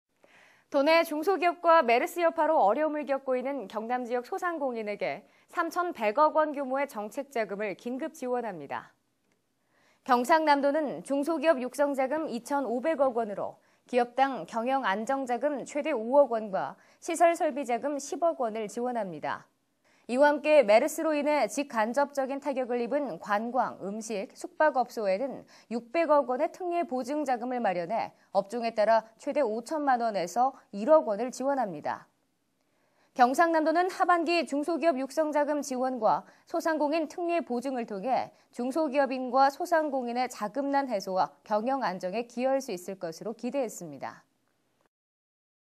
메르스 피해 소상공인에 3,100억 원 지원
도내 중소기업과 메르스 여파로 어려움을 겪고 있는 경남지역 소상공인에게 3,100억 원 규모의 정책자금을 긴급 지원합니다.

경상남도는 중소기업육성자금 2천 5백억 원으로 기업 당 경영안정자금 최대 5억 원과 시설설비자금 10억 원을 지원합니다.

이와 함께, 메르스로 인해 직・간접적인 타격을 입은 관광, 음식, 숙박업소에는 600억 원의 특례보증 자금을 마련해 업종에 따라 최대 5천만 원~1억 원을 지원합니다.

경남도는 하반기 중소기업육성자금 지원과 소상공인 특례보증을 통해 중소기업인과 소상공인의 자금난 해소와 경영안정에 기여할 수 있을 것으로 기대했습니다.